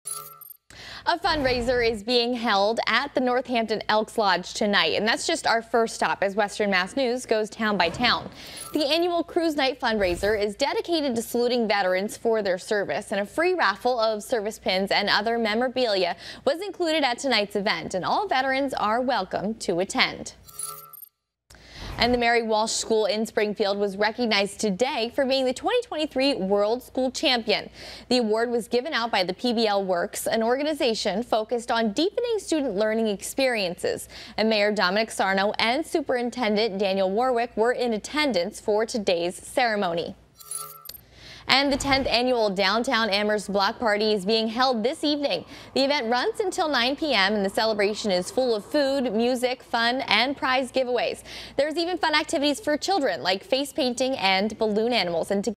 Town by Town: Elks Lodge Fundraiser, School Award, Block Party
Town by Town is taking you to Northampton, Springfield, and Amherst.